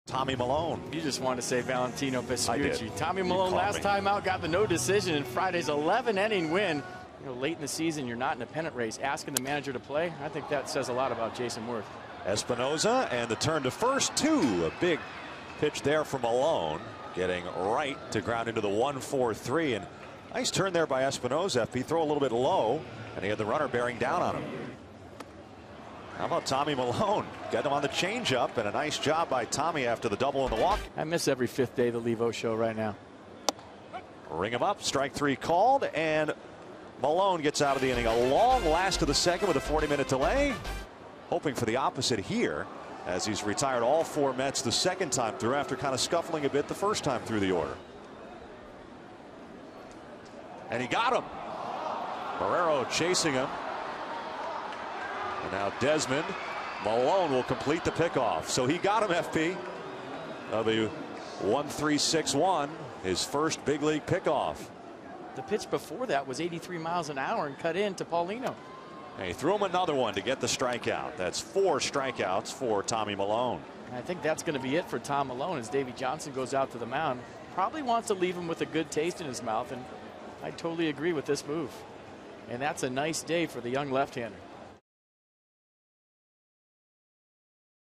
2011/09/15 Milone's first career win
WSH@NYM: Milone earns his first Major League victory